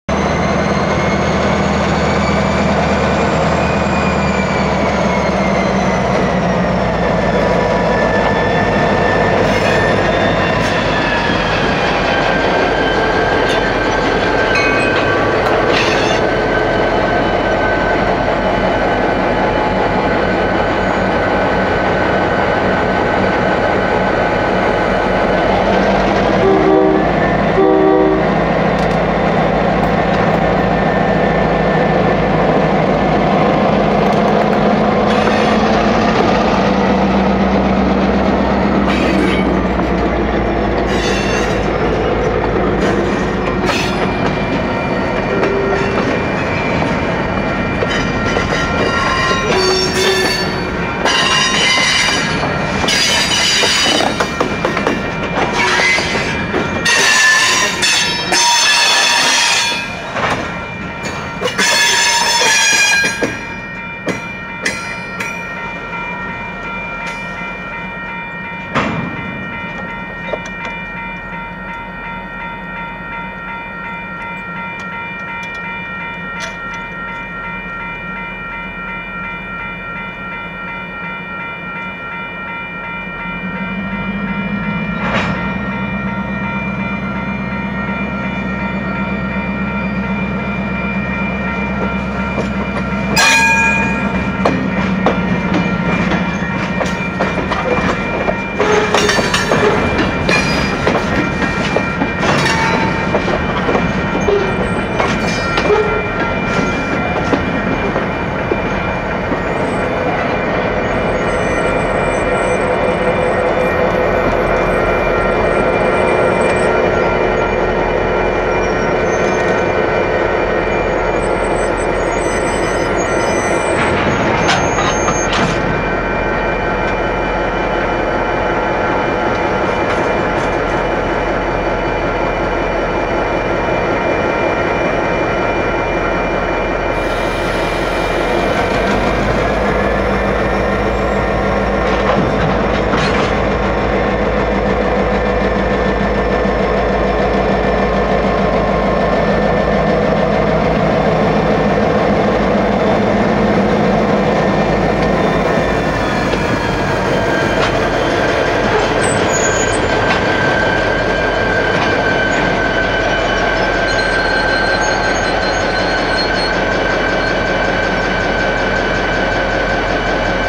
Fallon 2006
In September 2006 on my first trip to USA drove from California into Nevada and called in at Fallon and by chance found UP 9989 and SP 574 busy marshalling wagons prior to departing for Sparks. An excellent start!